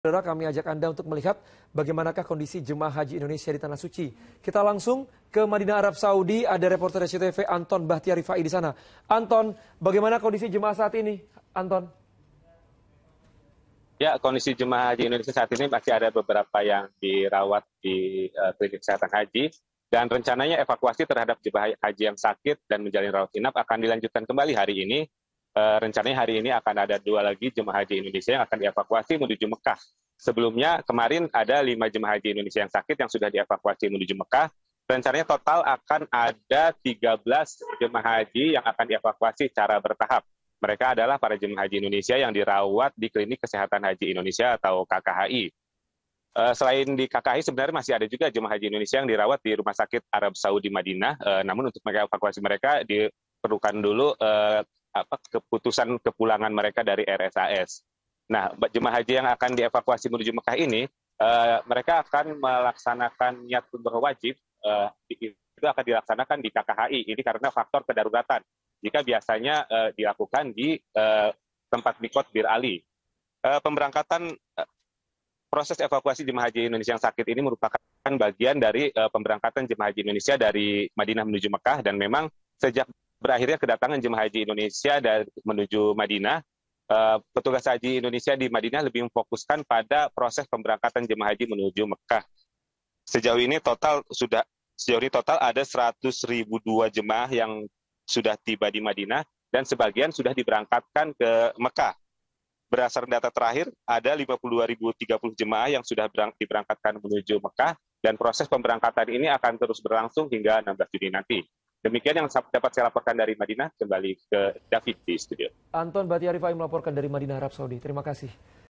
Live Report: Jemaah Haji Asal Indonesia yang Sakit di Madinah Dievakuasi ke Makkah | Liputan 6
Live Report: Jemaah Haji Asal Indonesia yang Skait di Madinah Dievakuasi ke Makkah | Liputan 6